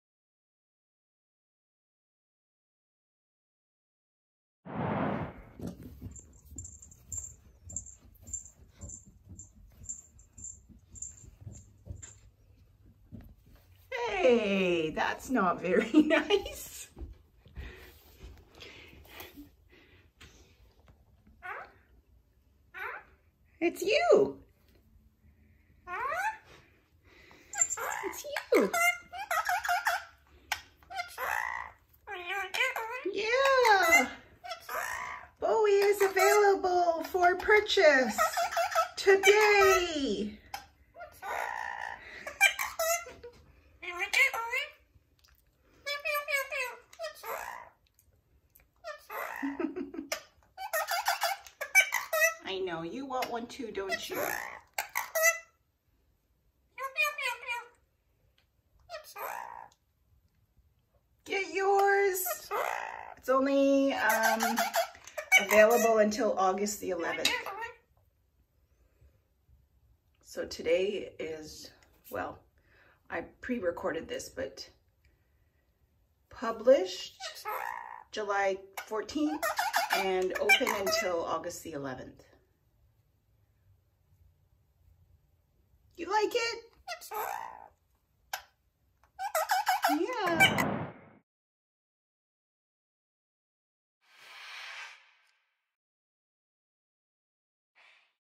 Tis The Season of Bowie Plush
It’s the season of Bowie Plush🐥

We need 200 orders for the Bowie Plush to go into production. Help us reach our goal!